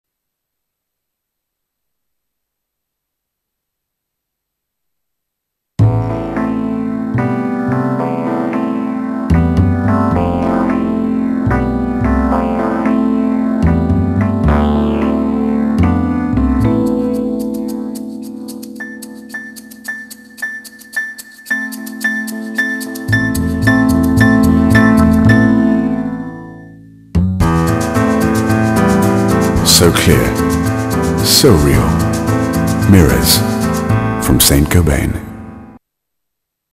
Saint-Gobain mirrors - Museum movie
Saint-Gobain Glass has a state-of-the-art range of mirrors made from the clearest kind of float glass in the world. Totally environment-friendly, these mirrors are characterized by excellent clarity, distortion-free reflection and high durability.